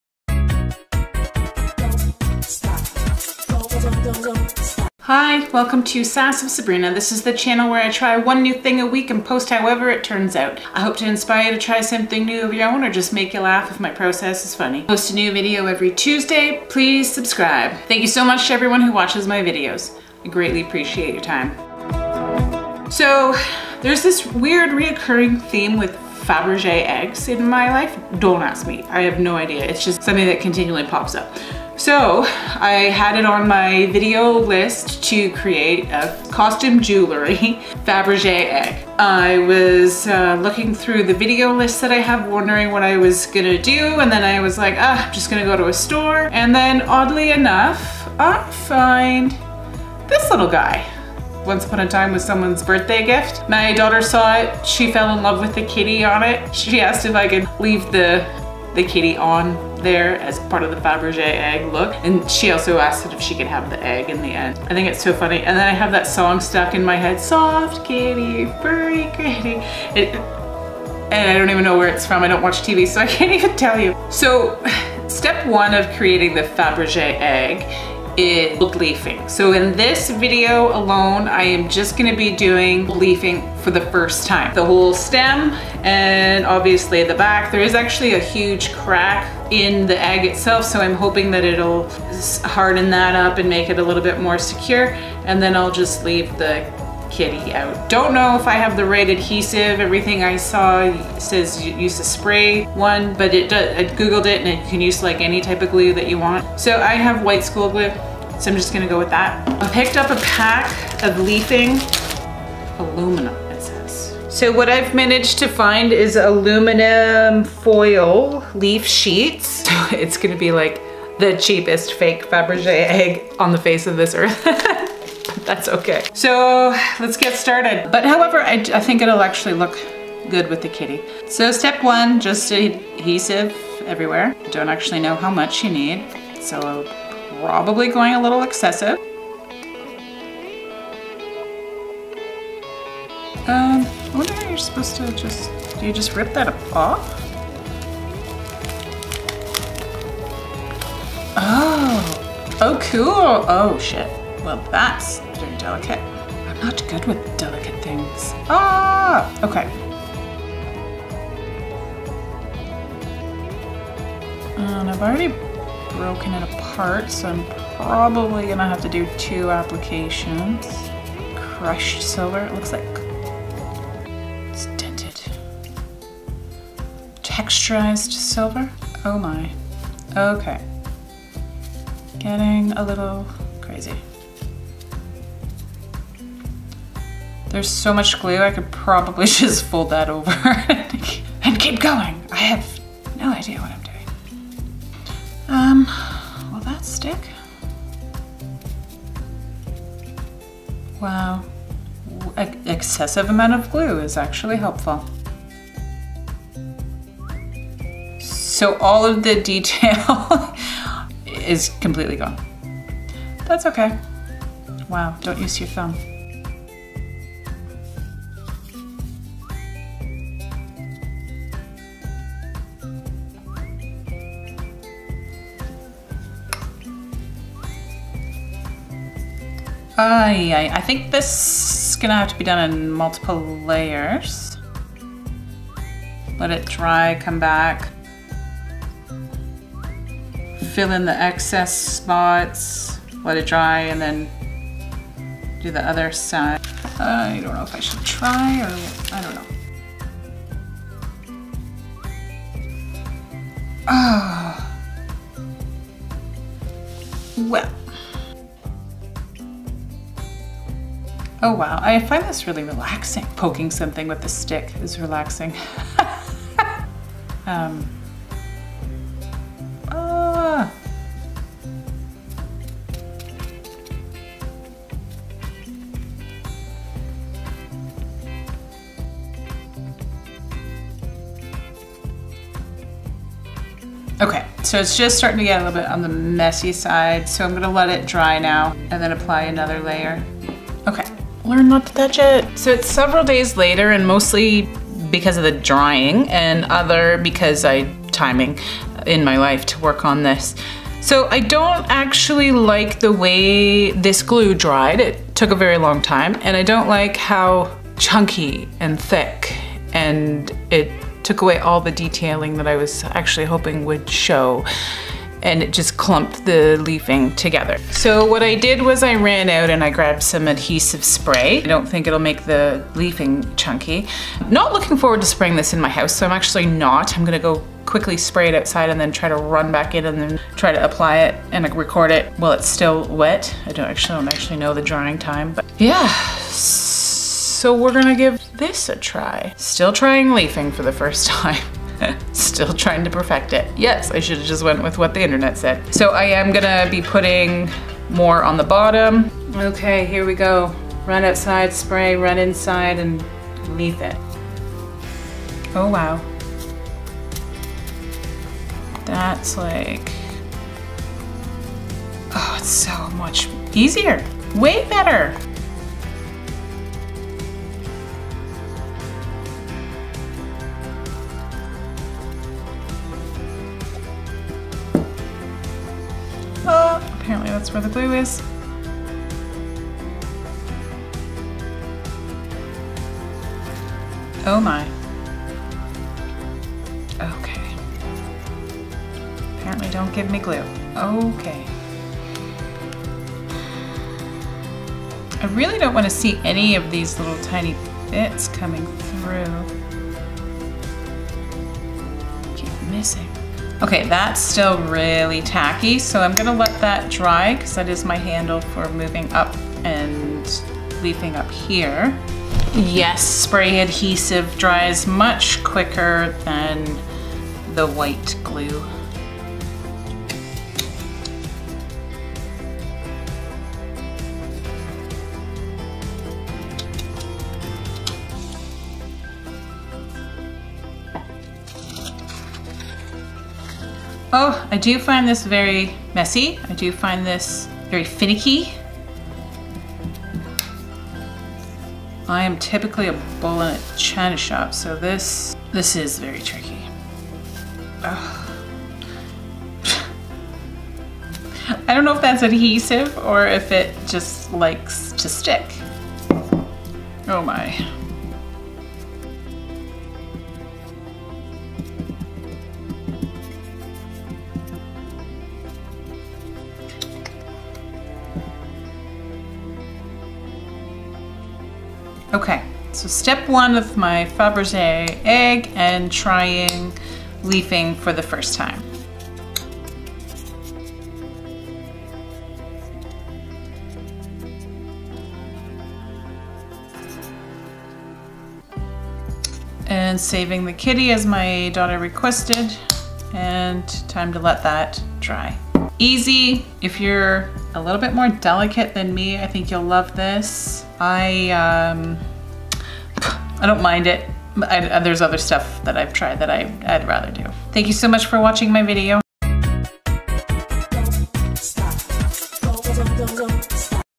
leafing for the first time
This is step one of creating a very knock-off faberge egg. I lucked out and found an egg on a stand to use as my base. My daughter liked the picture on it so we are keeping that showing and leafing the rest.  This video is the results of trying leafing or the first time.

Donations are greatly appreciated it helps me to keep trying stuff!